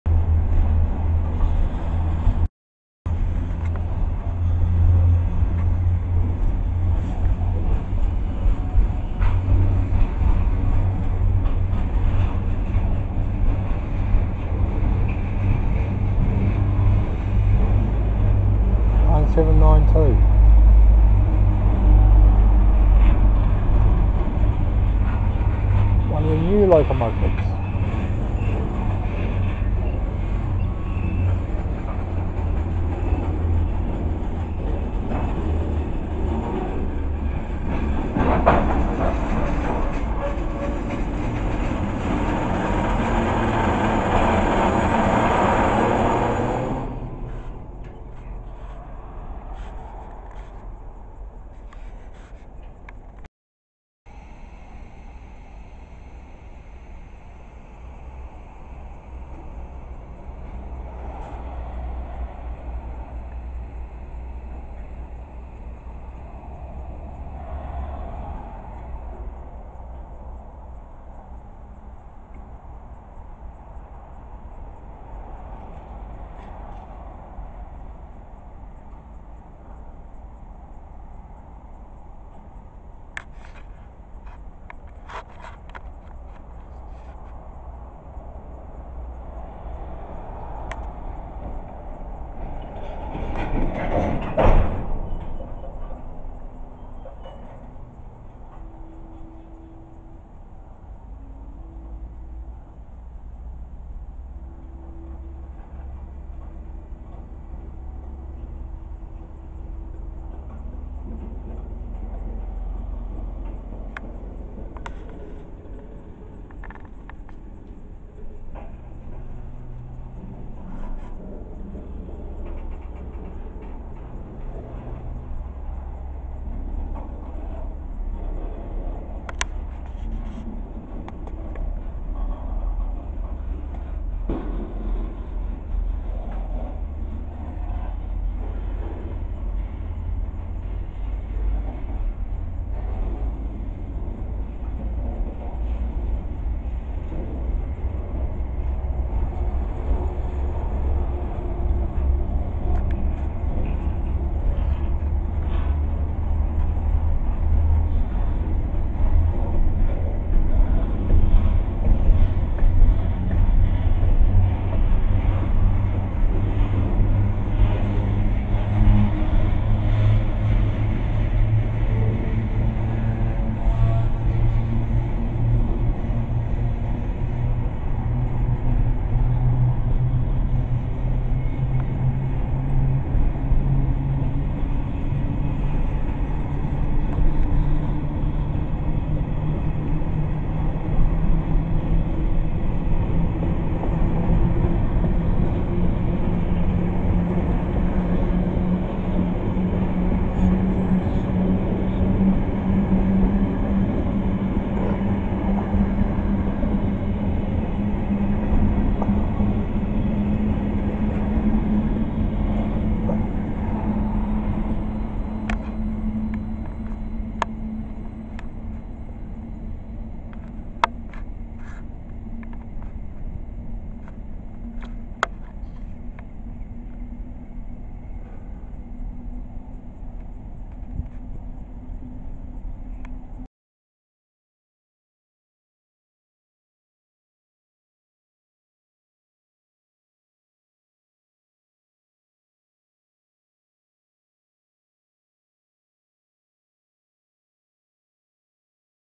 Container Train heading South pulled by DL9792 , Huntly 14th June 2023
I had just packed up everything when this came through and missed the Locomotive .